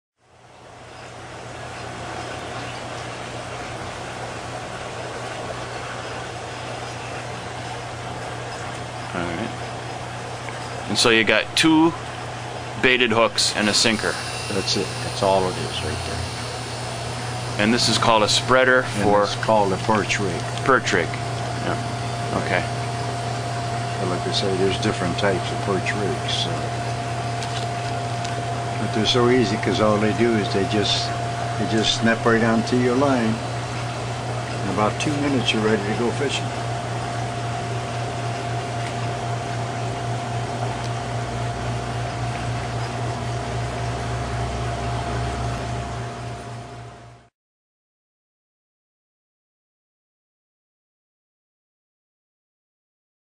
Perch Rig for Lake St. Clair
How to catch perch in Lake St. Clair using a "spreader-style" perch rig